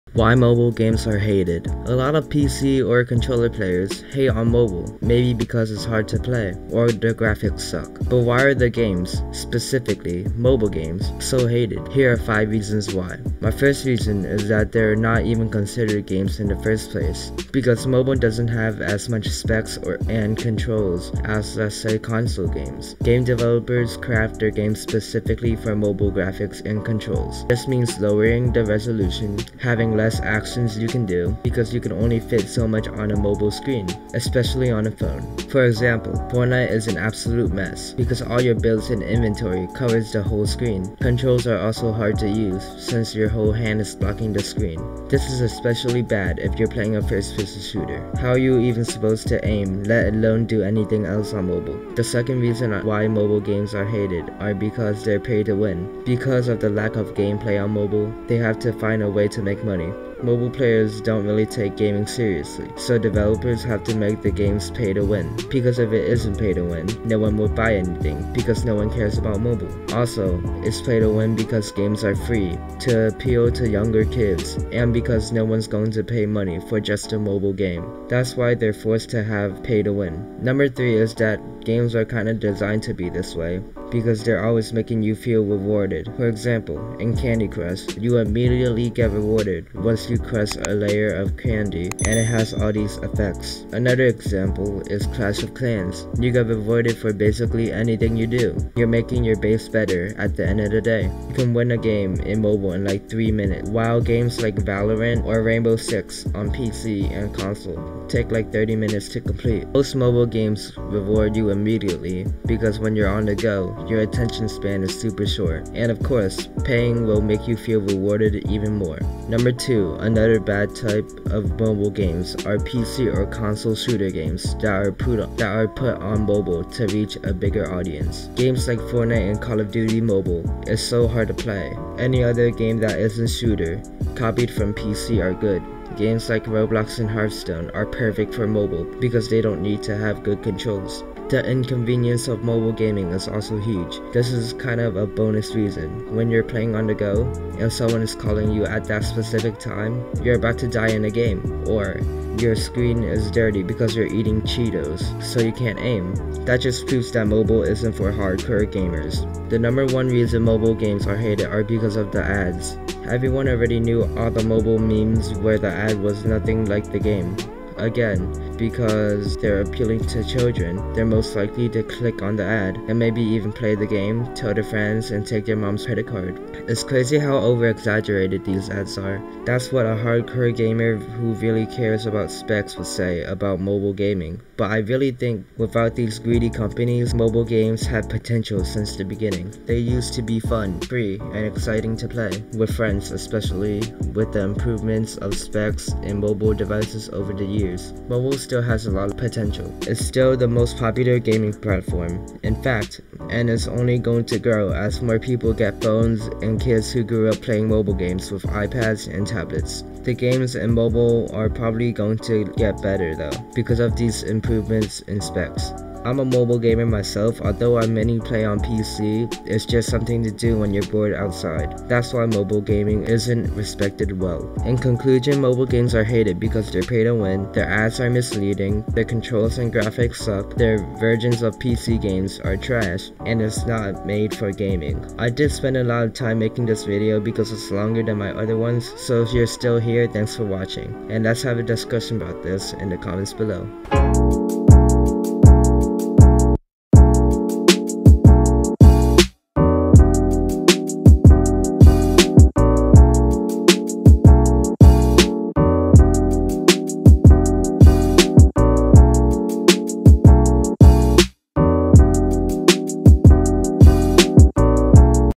Why Mobile Games are HATED // why does mobile gaming get so much hate?
Why mobile games are hated. Mobile games get a ton of hate because they're not as effective as pc or console gaming. They haved harder controls and worse grapics. Gamers understand that, but the reason they're hated is because of all the bad things big companies do to children. They make games pay to win, because the games are free and you get so much rewards if you pay. Another huge problem is that they're advertisements are trash. Mobile ads are nothing like the game. Basically, Mobile gaming is hated because companies appeal to children, and the limits of mobile specs.